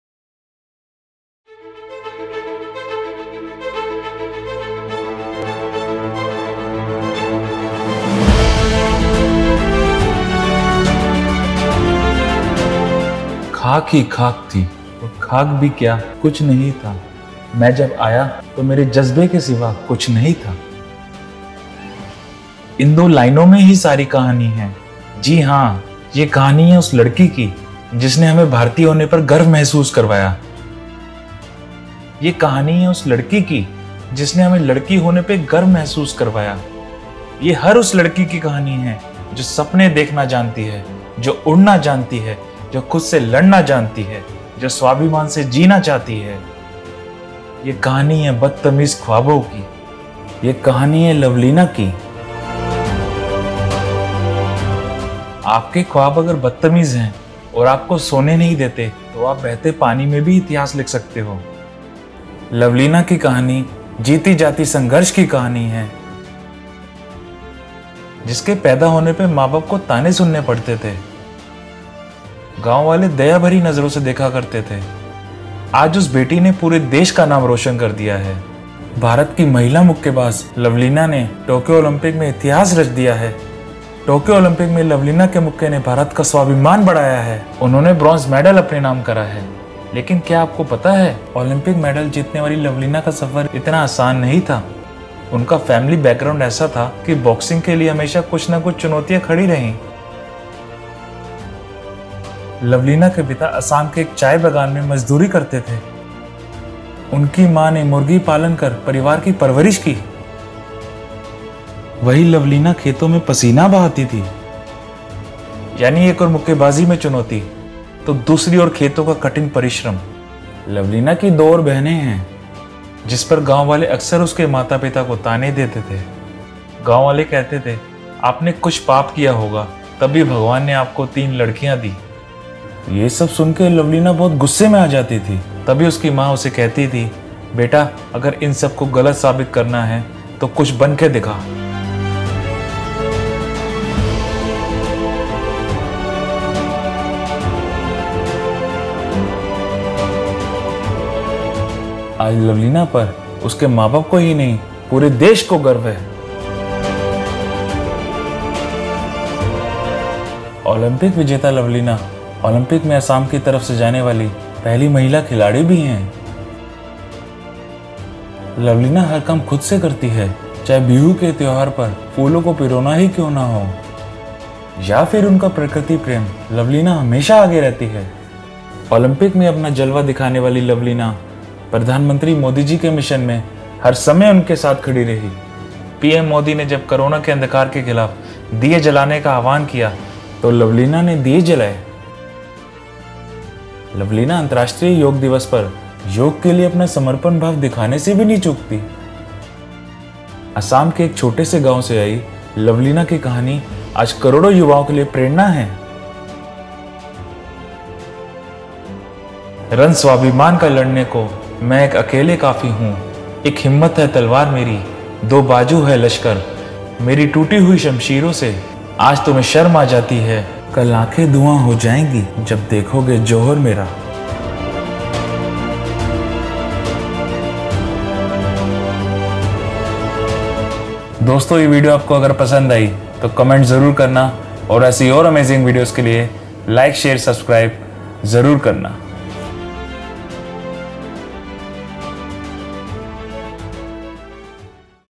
Lovlina Borgohain Motivational Story 🔥 Villager to an Olympian 🔥 Tokyo Olympics 2020 | Manish Arora
While the country celebrated Boxer Lovlina Borgohain’s assurance of a bronze medal, Baro Mukhia, Lovlina's village,  held a picture of neglect inundated by heavy rainfall. However, the locals claim that the Assam government has started developmental work at the village after her performance.
In this video, we discover the motivational story of Lovlina Borgohain. Lovlina Borgohain, who had already win a bronze medal, lost the 69kg women's boxing semifinal bout against reigning world champion Busenaz Surmeneli of Turkey at the Tokyo Olympics.
This story is full of motivation, struggle, and victory!